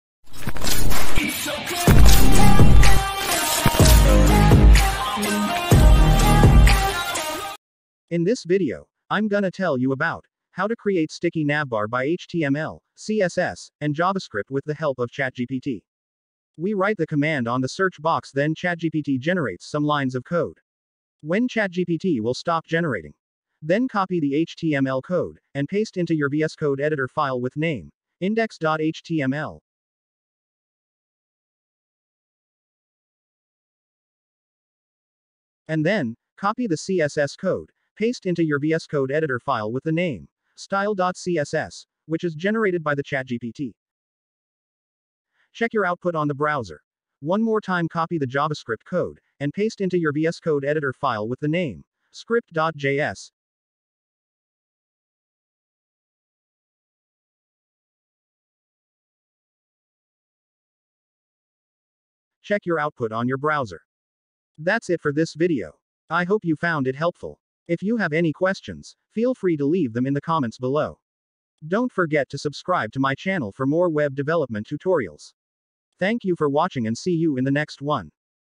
Build Sticky Navbar in 1 minute by you using ChatGPT
In this video I'm gonna tell you about how to build Sticky Navbar in 1 minute by you using ChatGPT. I am also gonna tell you about ChatGPT that how we can use or access it with which you can generate or solve many of the programming issues. Build Sticky Navbar in 1 minute by you using ChatGPT
⚫Background music